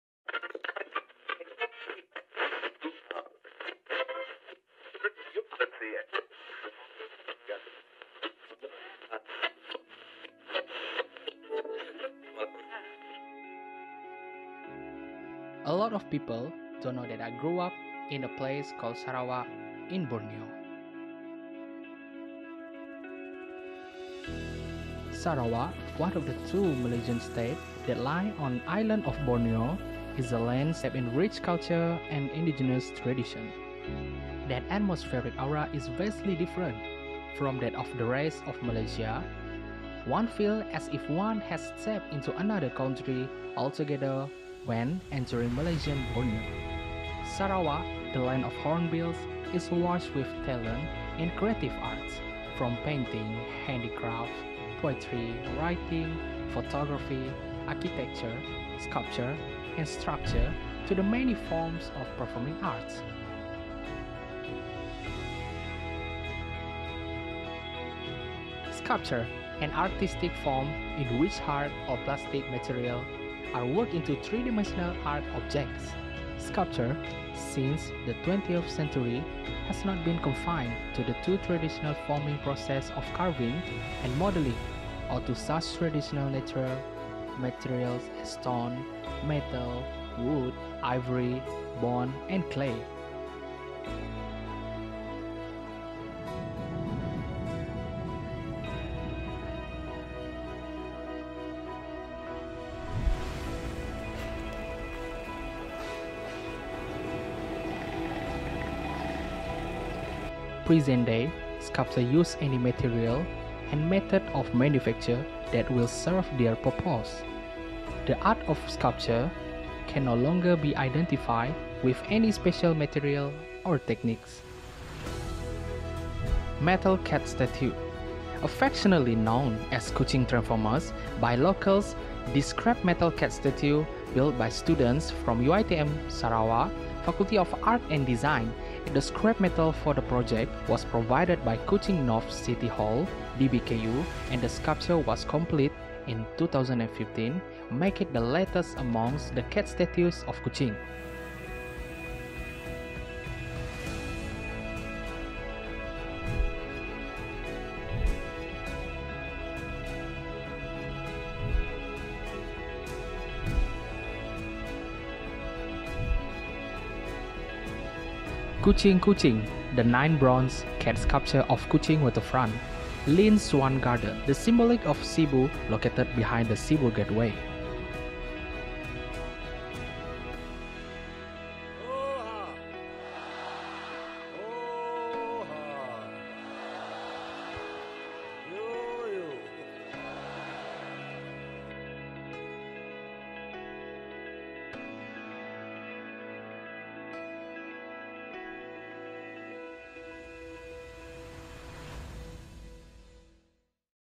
The Soul of Borneo | Sculpture in Sarawak (Pustaka Negeri Sarawak) 2019
Video title:

"The Soul of Borneo"

Group members:

Mohd Shahmin Bin Sahari
Bartolomew Andoy Anak Antas

Video synopsis:

Southeast Asian is heavily influenced by its religion, cultural beliefs and traditions. In Malaysia, Sarawak is one of the state in Malaysia to have more than 27 ethnic groups (indigenous), each with their own languages, culture and beliefs. There are Iban, Bidayuh, Melanau, Malay, Bisaya, Orang Ulu, Penan etc. The indigenous people are special, they are writers, performers, and artists, and they hail from diverse indigenous communities worldwide. Sarawak is awash with talent in creative arts from painting, handicrafts, poetry, writing, photography, architecture, sculpture, and structure, to the many forms of performing arts. As a Sarawakian and for the love of our Motherland, we have had the honor to produce this video. Theme ‘Sculptures in Sarawak’ we wanting to show visitors what is the special about our own local arts especially art of sculpturein Sarawak. In this video our main focus is to show some of the main statues with some information and history about it that found in Kuching and other places like Sibu.

Full Edit by Shahmin Sahari